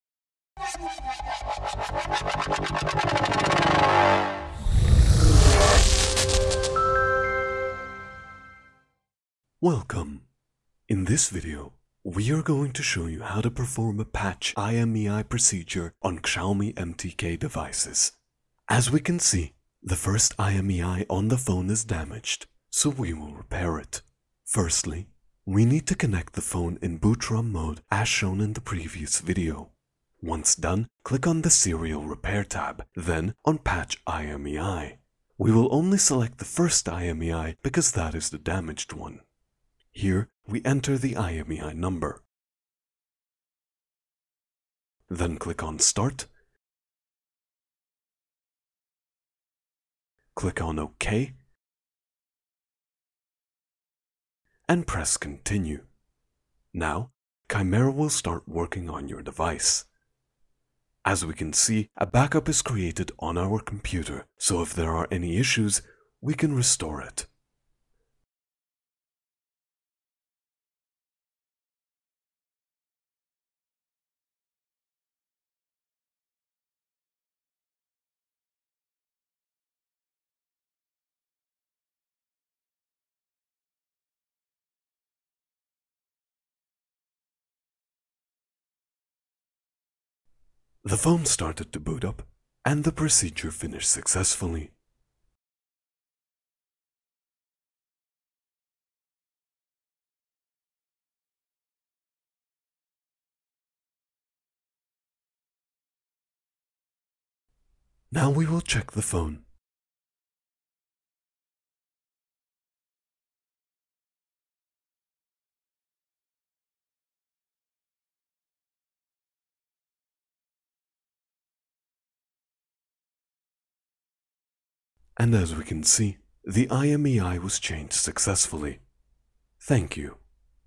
Xiaomi MTK Patch IMEI with ChimeraTool
In this video, we are going to show you how to perform a Patch IMEI procedure and repair a damaged IMEI number on your Xiaomi MTK devices using ChimeraTool.